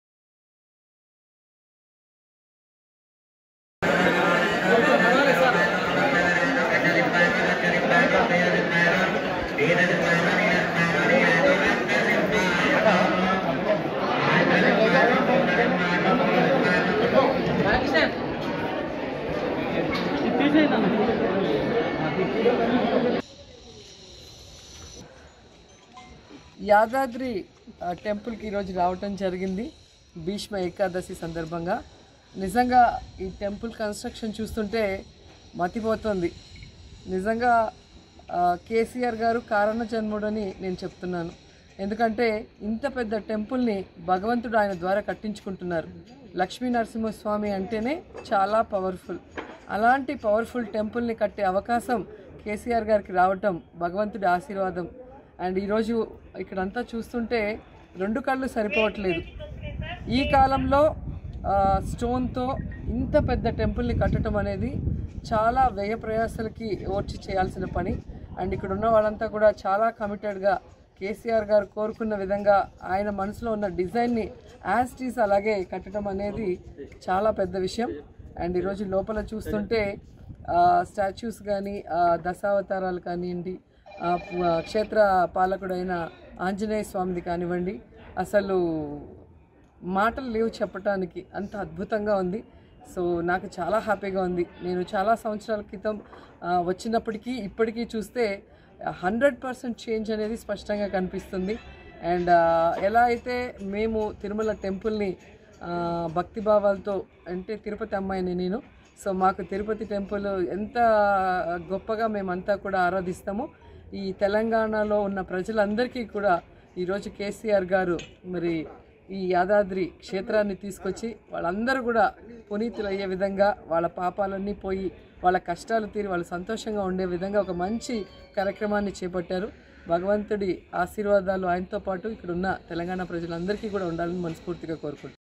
యాదాద్రి శ్రీలక్ష్మీనరసింహస్వామి వారిని దర్శించుకున్న ఎమ్మెల్యే రోజా.. || Silver Screen
Watch : యాదాద్రి శ్రీలక్ష్మీనరసింహస్వామి వారిని దర్శించుకున్న ఎమ్మెల్యే రోజా.. || Silver Screen